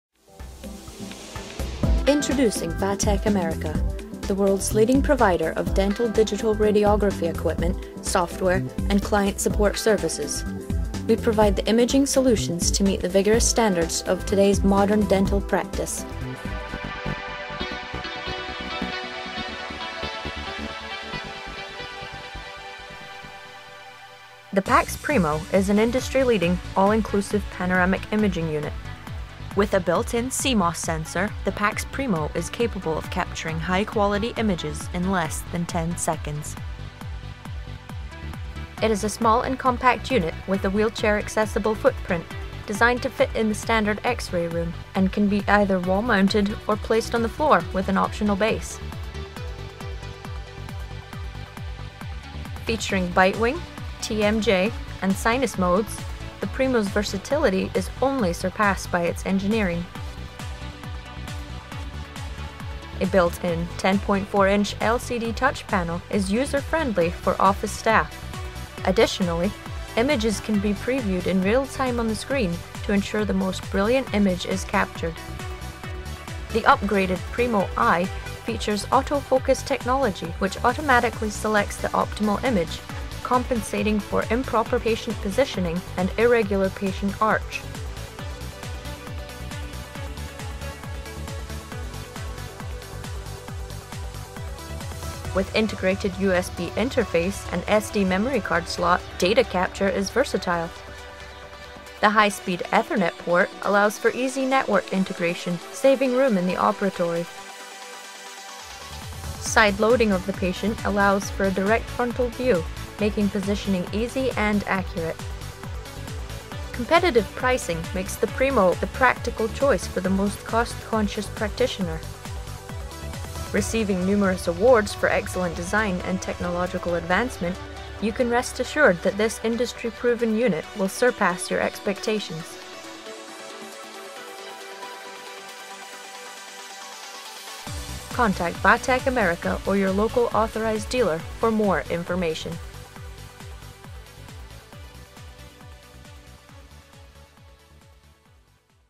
PaX-Primo Video.wmv
Equipped with a new generation of CMOS digital sensor, PaXPrimo delivers diagnostic panoramic x-ray images equal to digital photo quality. Also available is the PaX-Primo(i) with optional Automatic Layer Selection Algorithm functionality to... produce optimal digital diagnostic images by using VATECH's proprietary Area Adaptive Mode (AAM) solution. The AAM solution captures and instantly processes multiple oral trough layers during a single panoramic scan. It intelligently reproduces a final diagnostic image by selecting the optimal single layer in different regions of the image to minimize image distortion in every panoramic image taken.

Both the PaX-Primo and PaX-Primo(i) include a high resolution 10.4" LCD touch panel for easy operation and simple image previewing. The very compact size and light weight of the PaX-Primo family (less than a 4' cube, and weighing less than 165 pounds), and the ease of installation will be appreciated by both dentists and dealers.